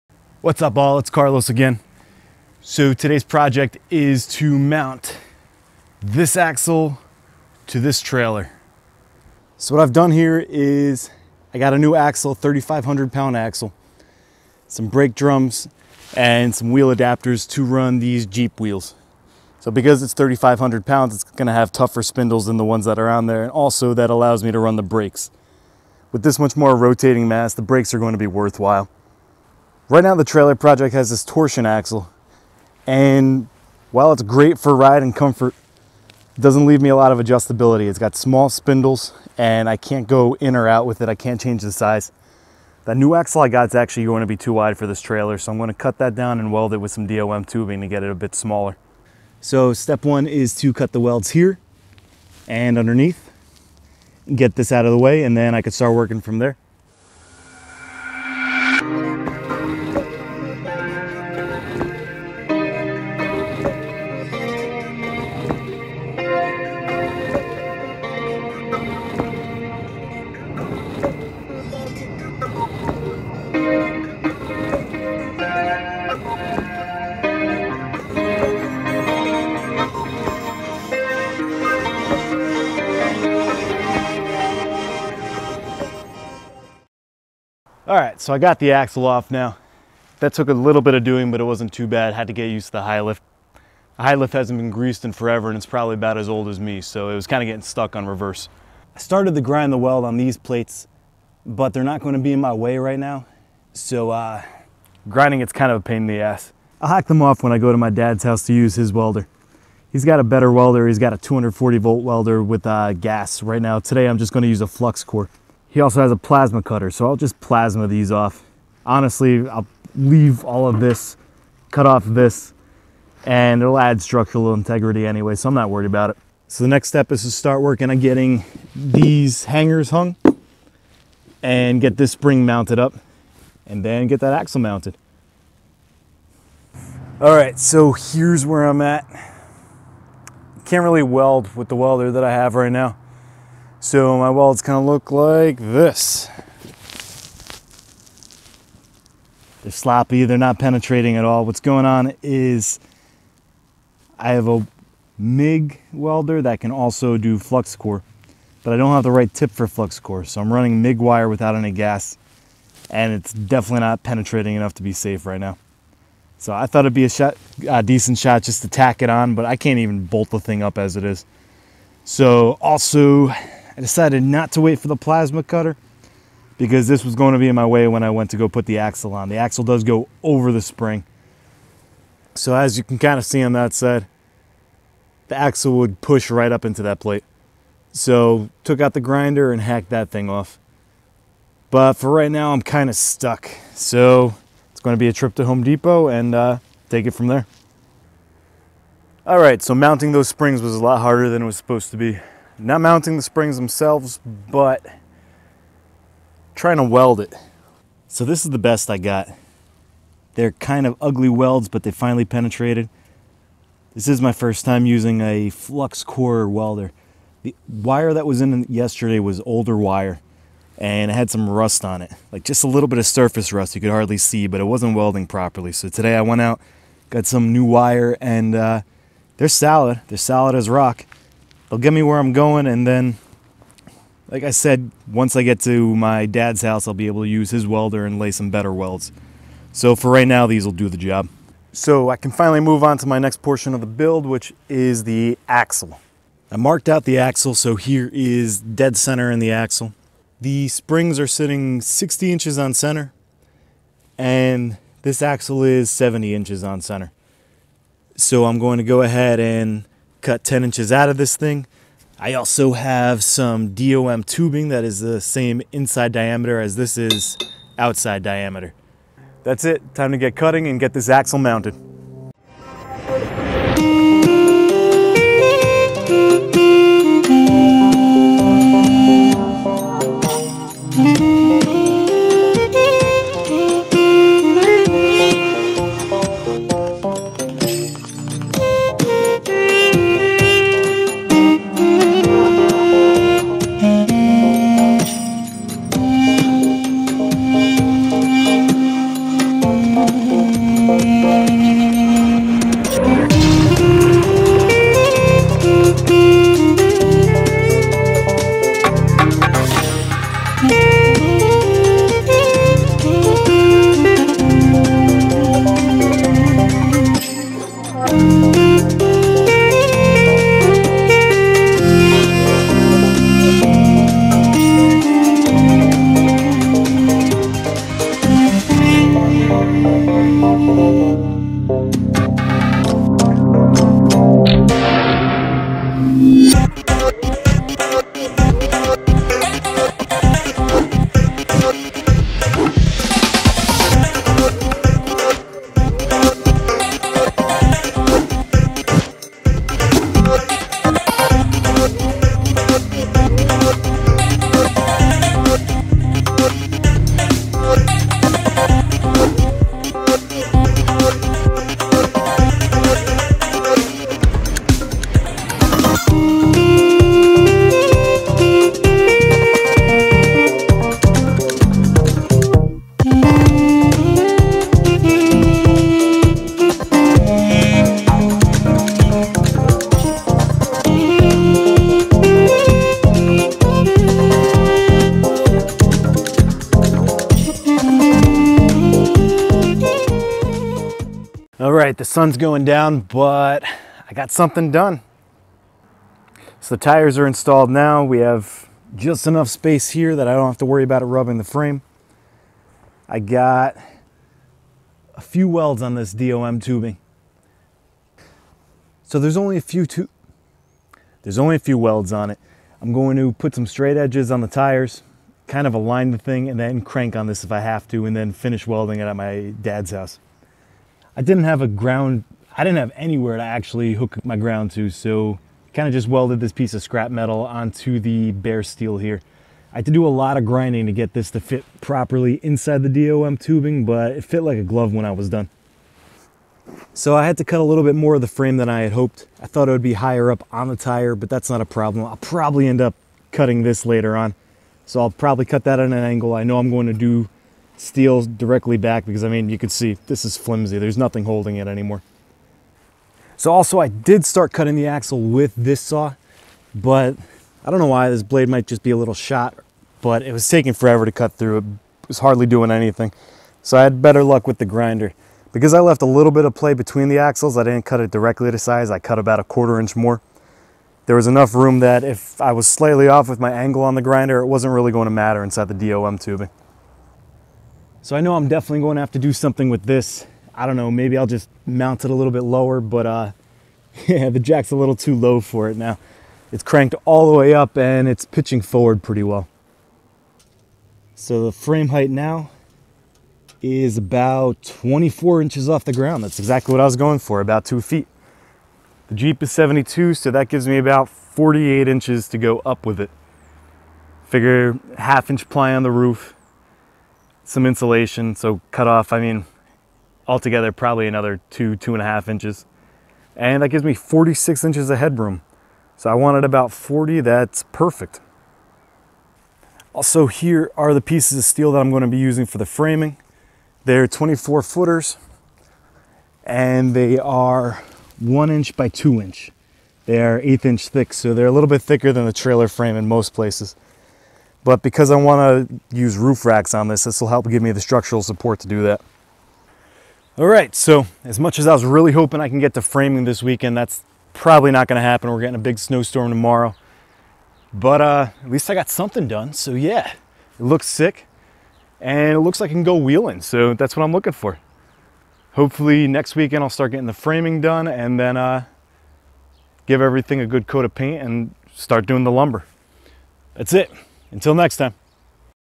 Adventure Trailer Build Part 2: Axle Install
The new axle brings the top of my frame about two feet off the ground.  What a pain it when you don't have the right tool for the job (i.e. CO2 Gas)